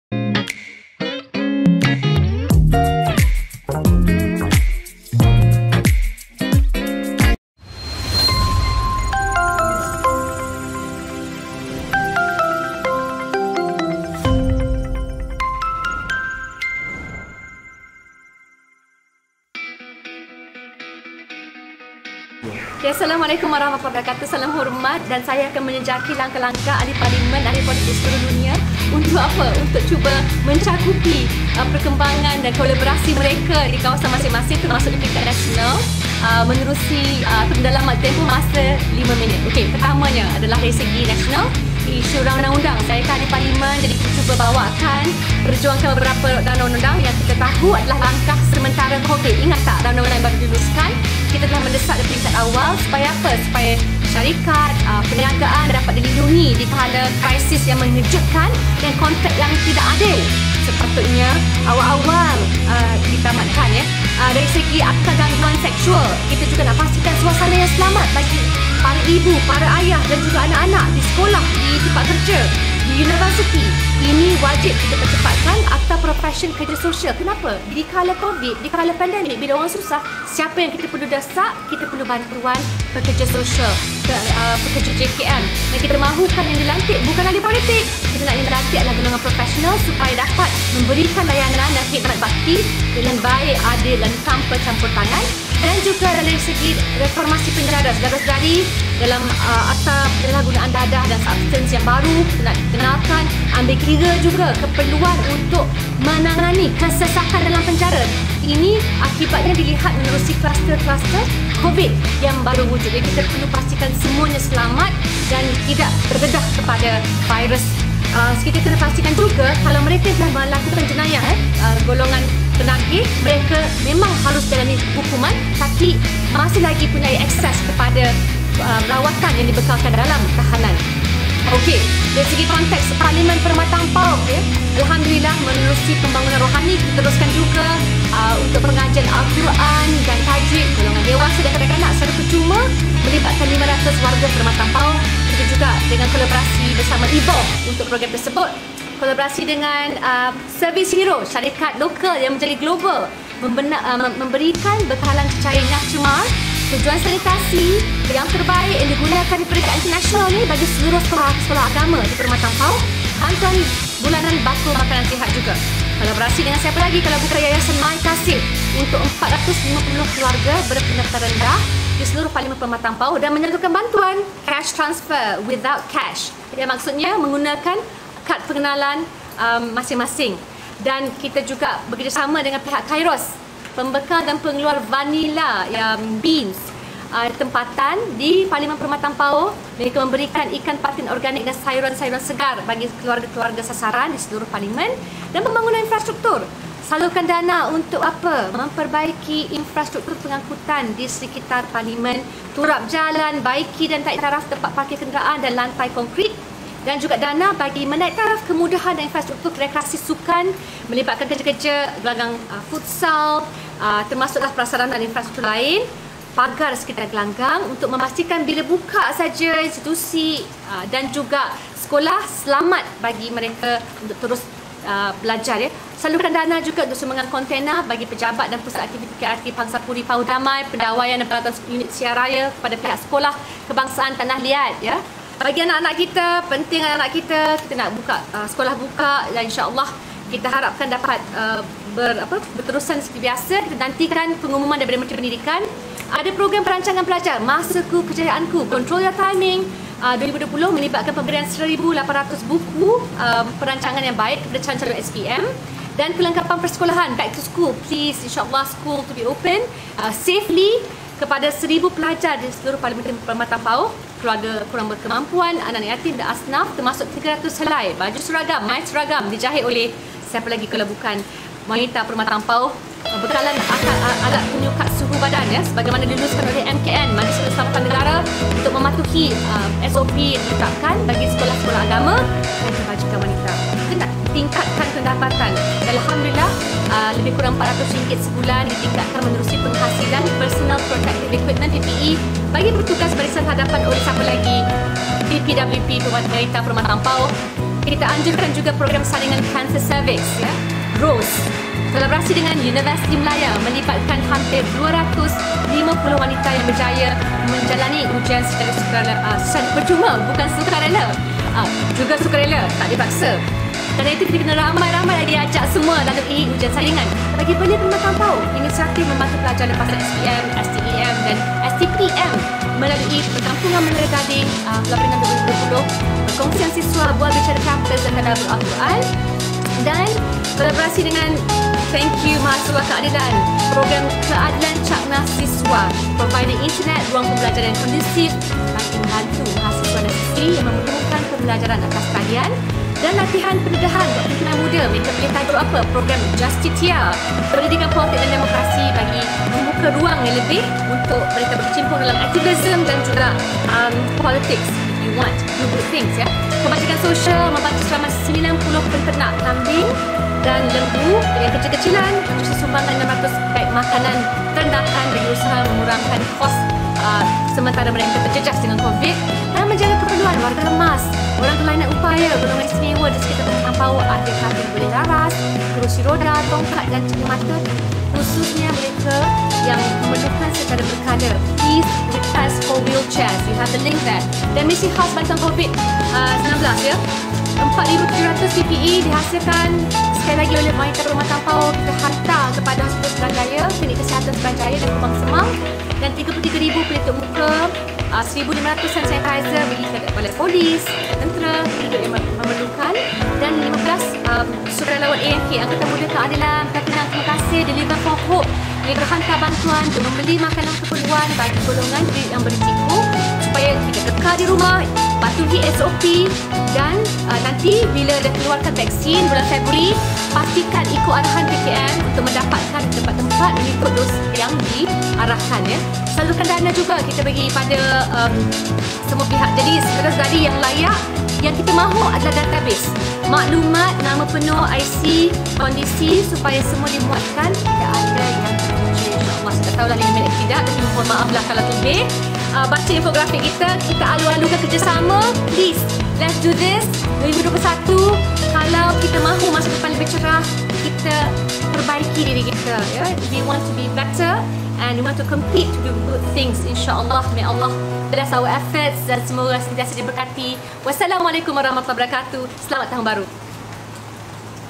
n_Izzah - PERMATANG PAUH 2020 DALAM MASA 5 MINIT!
Alhamdulillah, saya rangkumkan antara kerja-kerja pelaksanaan pasukan KEADILAN di Permatang Pauh sepanjang tahun 2020. Segala usaha ini tidak akan mampu kami laksanakan tanpa sokongan daripada warga Permatang Pauh yang terdiri daripada MPKK, JPWK, Anggota Cabang serta Ranting, AMK dan semua sukarelawan! Semoga 2021 ini kami dapat terus berkhidmat dan melaksanakan tanggungjawab demi kesejahteraan warga setempat, InshaaAllah!